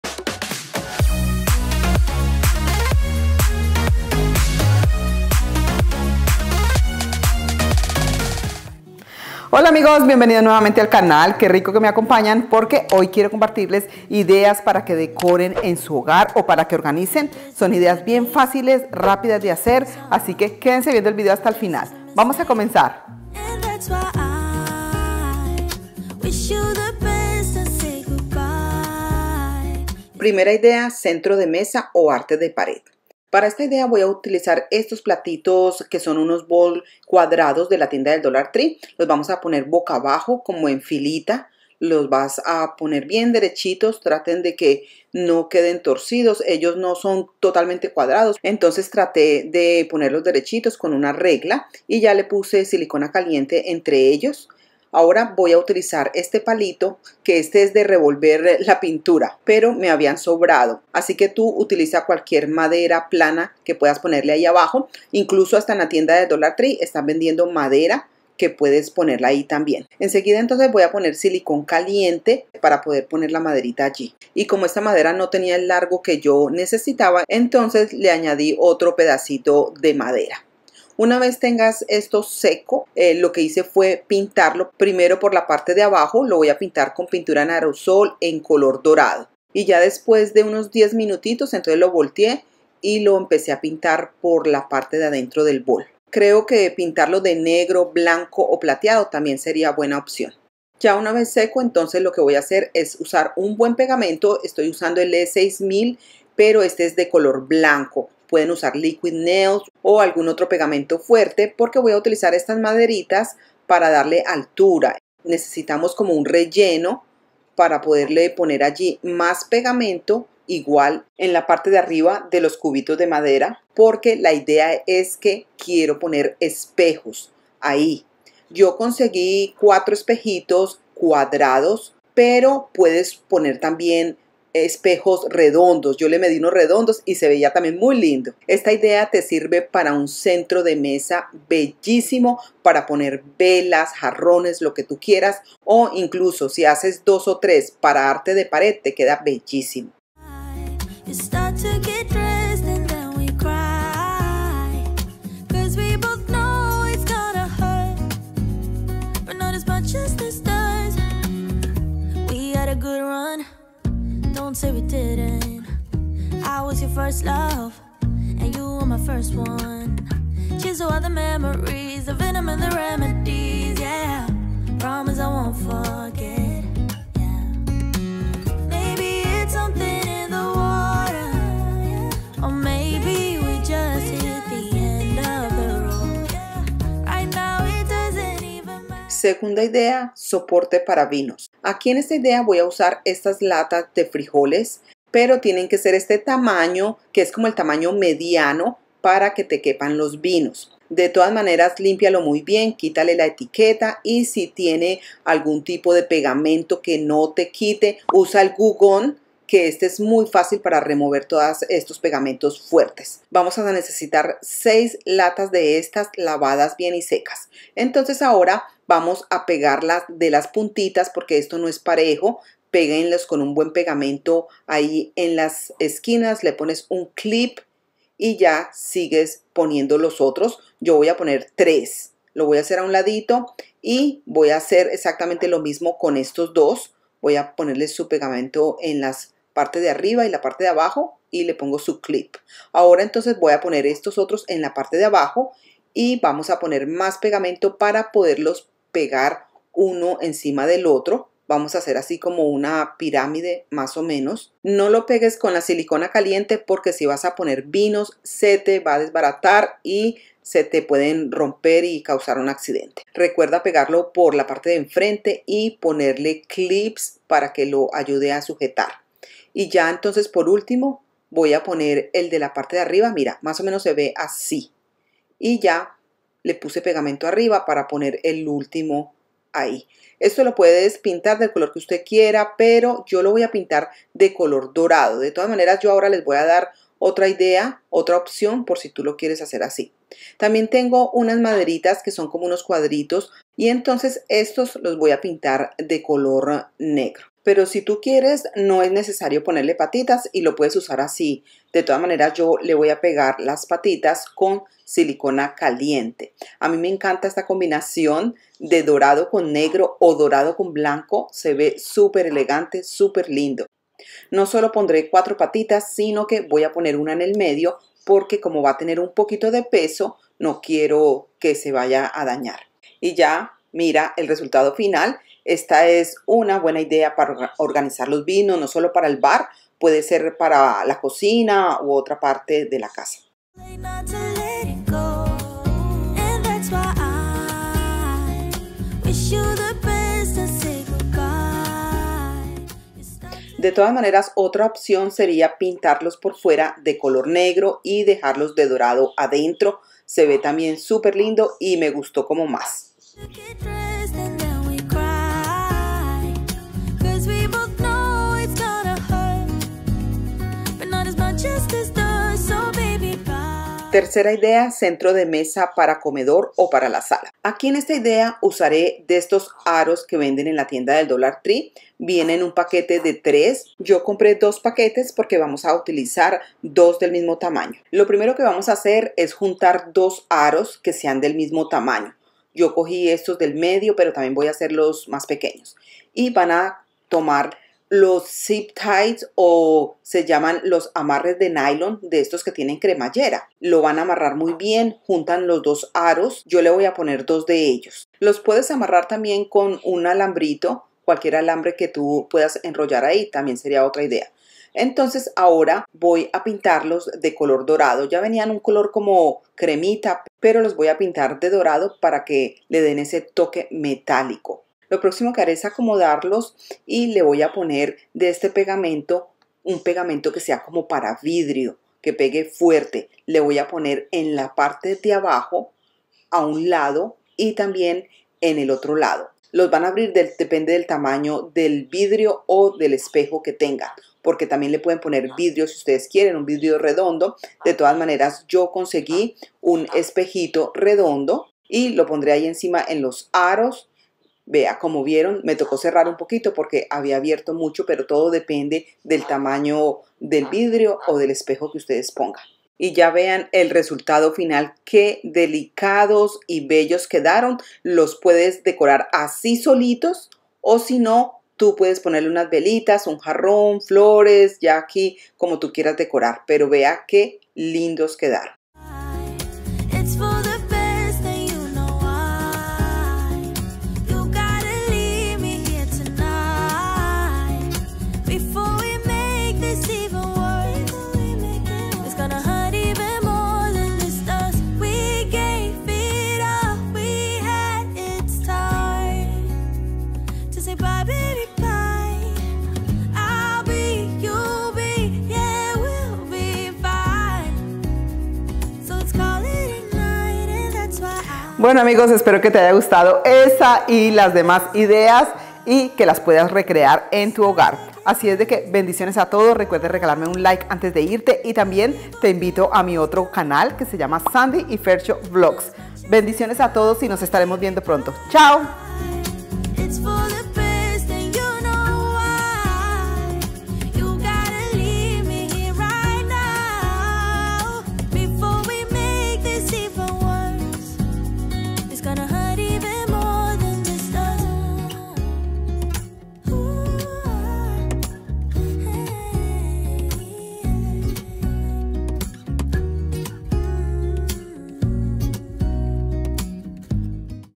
COMO HACER CENTROS DE MESA LINDOS FACILES Y ECONOMICOS
Te comparto cómo hacer centros de mesa y un porta vinos facil, sencillo y funcional.

✅Mi direccion por si quieres enviarme algo
United States